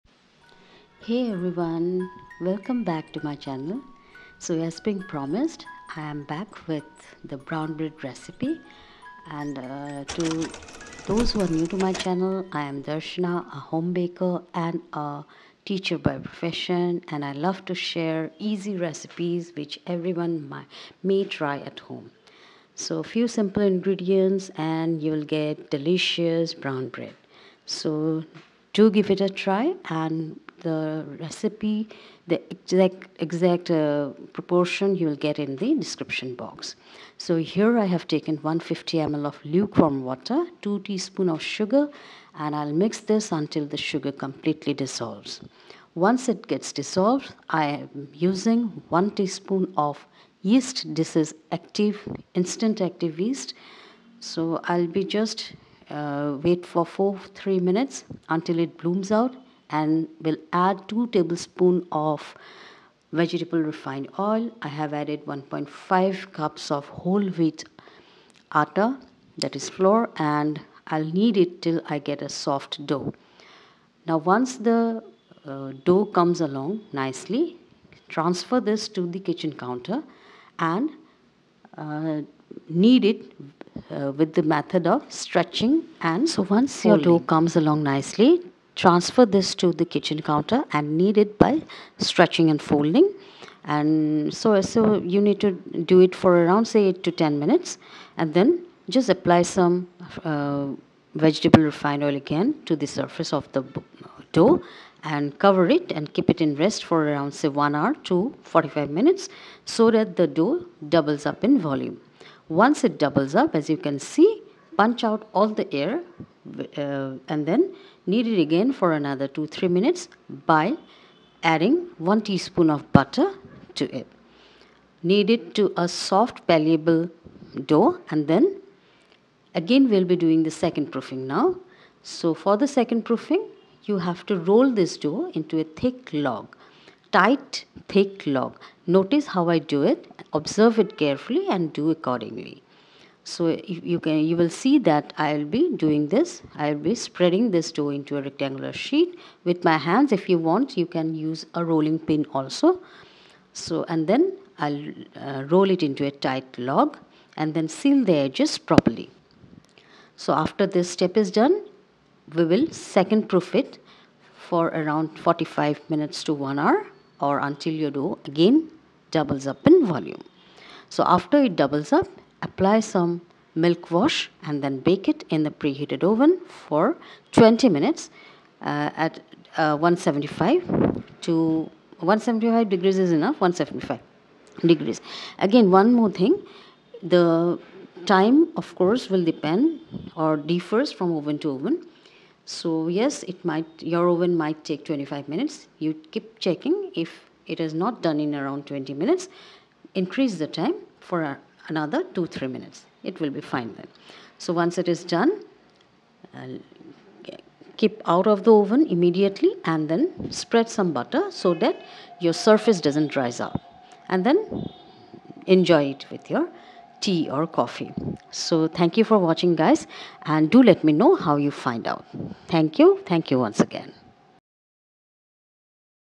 Brown Bread | Homemade Brown Bread | Aata Bread| Easy Healthy Bread | Whole Wheat Bread
Ingredients:
~whole wheat aata 1.5 cups
~ 150 ml luke warm water

** Please go through the video properly, all points are explained in details which
will help you to make this perfectly.

Do let me know how you find ❤️

~Duotone rosettes videos

~Perfect way to make stiff peak with whipped cream

If you like my recipes do follow me in the below mentioned handlers 😊

Music I used: